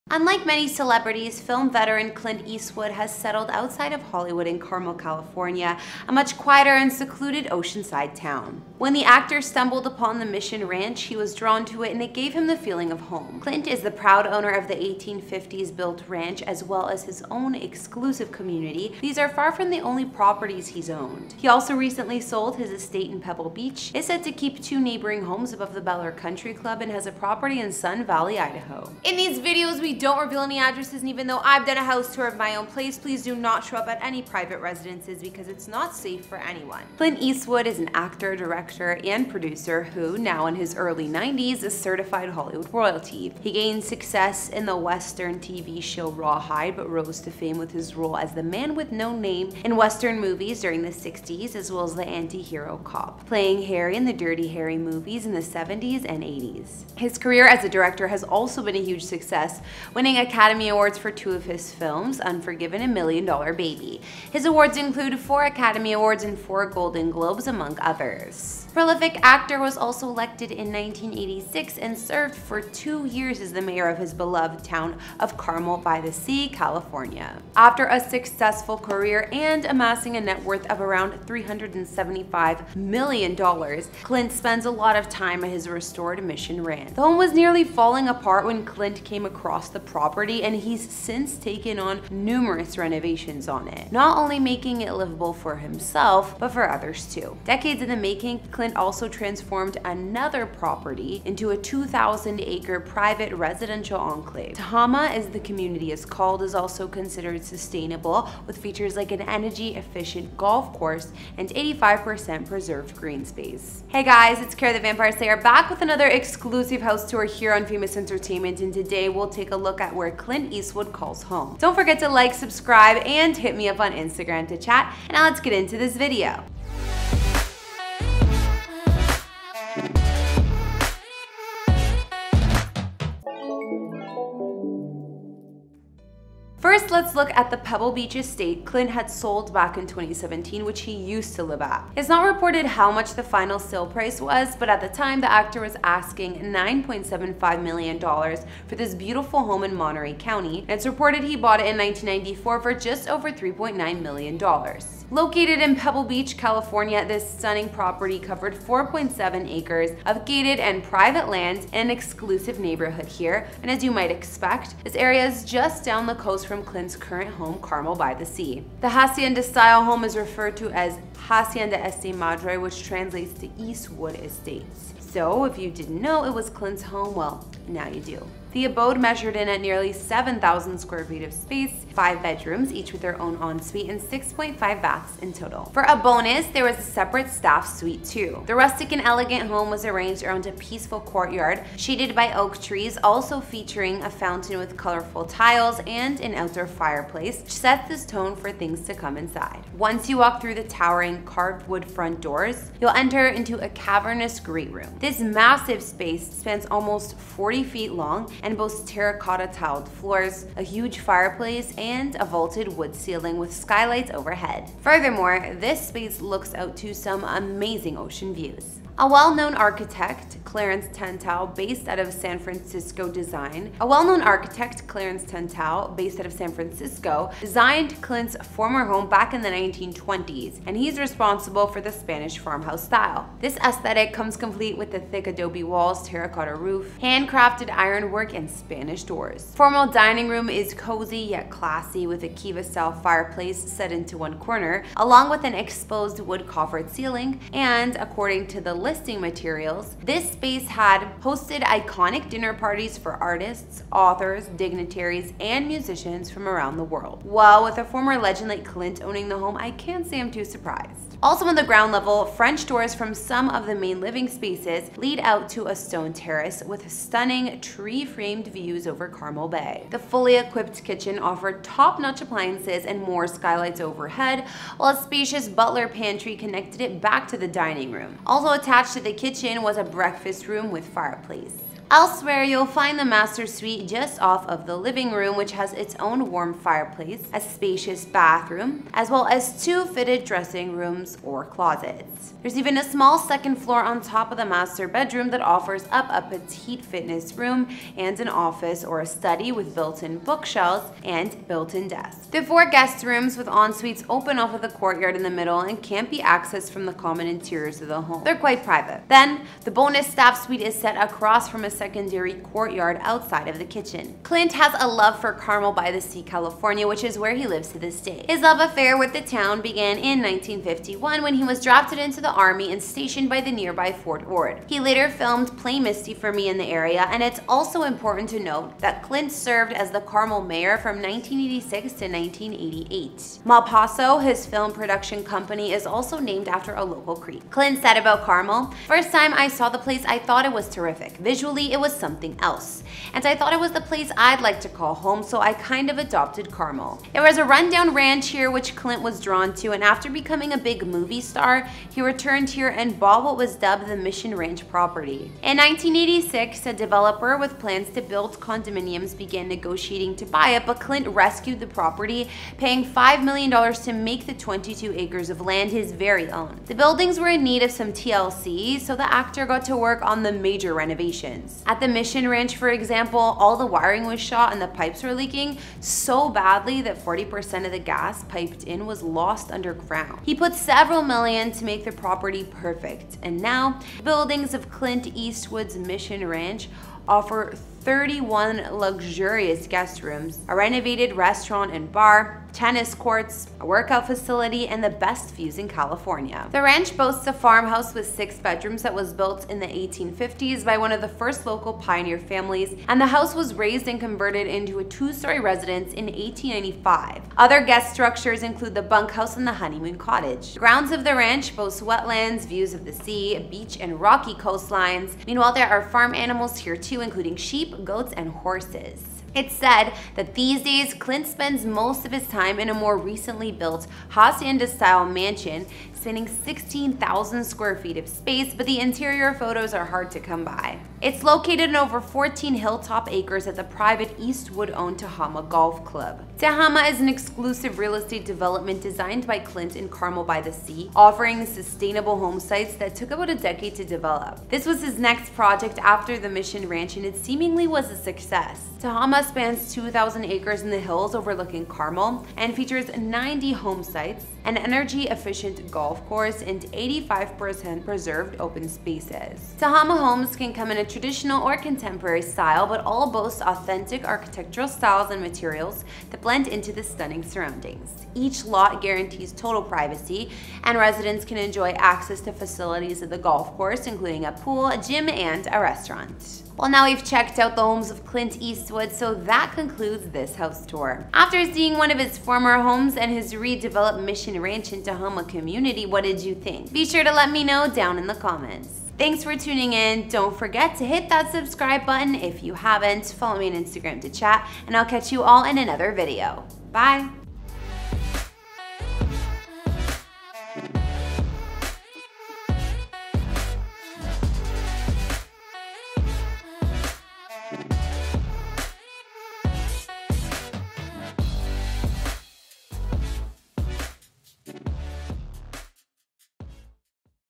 Clint Eastwood | House Tour | $10 Million Pebble Beach Mansion & More
Unlike many celebrities, film veteran Clint Eastwood has settled outside of Hollywood, in Carmel, California – a much quieter and secluded Oceanside town. When the actor stumbled upon the Mission Ranch he was drawn to it and it gave him the feeling of home. Clint is the proud owner of the 1850-built ranch as well as his own exclusive community but these are far from the only property he’s owned. He also recently sold his estate in Pebble Beach, is said to keep two neighboring homes above the Bel Air Country club, and has a property in Sun Valley, Idaho.